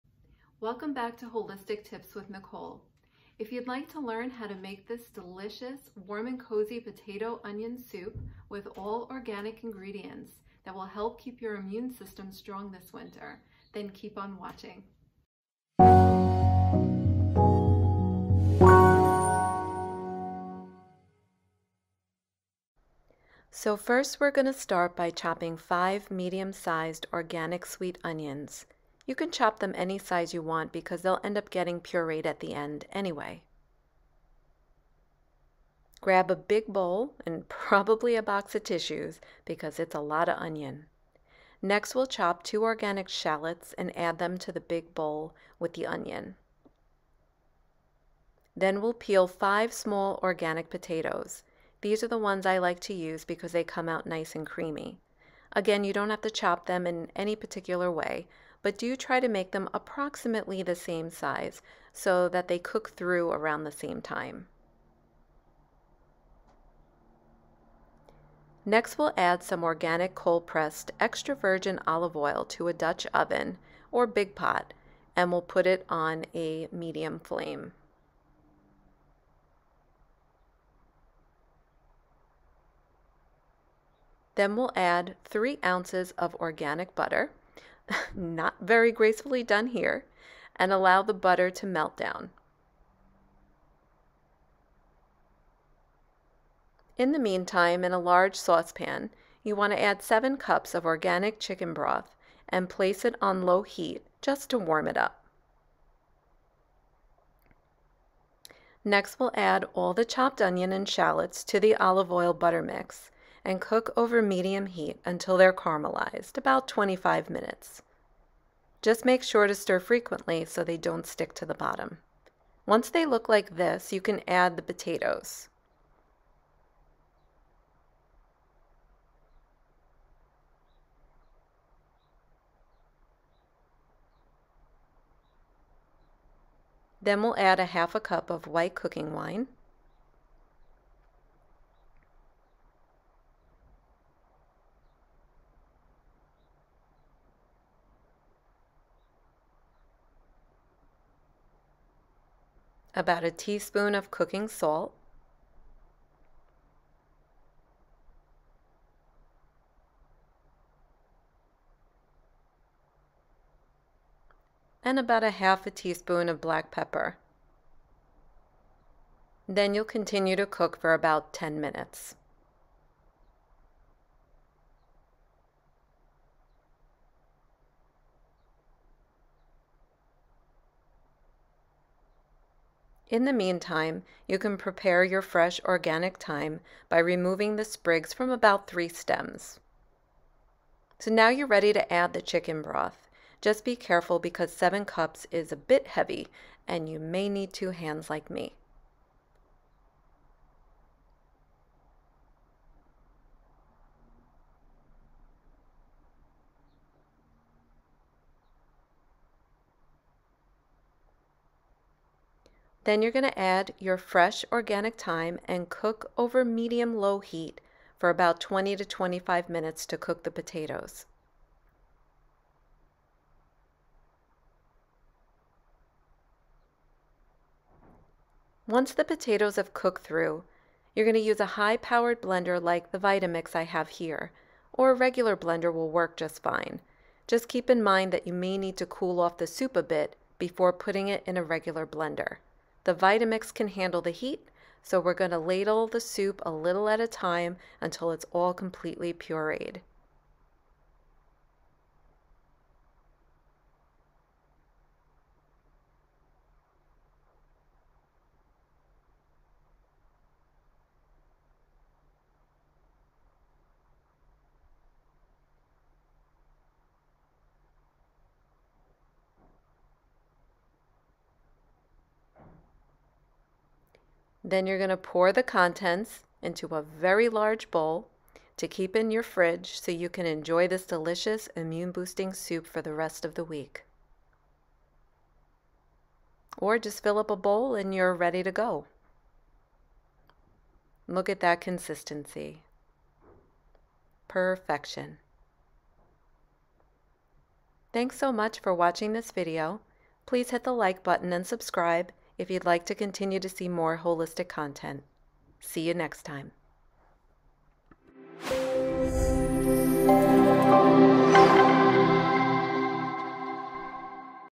Holistic Recipe: Organic Potato Onion Soup HD 1080p
Onions have been known to protect the body from viruses.  For a delicious, immune boosting organic onion soup that will warm you up this winter, please watch today's video.  Here are the ingredients:

• 5 medium size organic sweet onions, chopped
• 2 organic shallots, chopped
• 5 small organic potatoes, chopped
• 3 sprigs of organic fresh thyme
• 7 cups of organic chicken stock
• 1/2 cup white cooking wine
• 3 oz. organic, grass-fed butter
• 1/4 cup cold-pressed organic extra virgin olive oil

Nicole Libretti, RN
Masters of Science in Holistic Nursing
AHNA Board Certified Advanced Holistic Nurse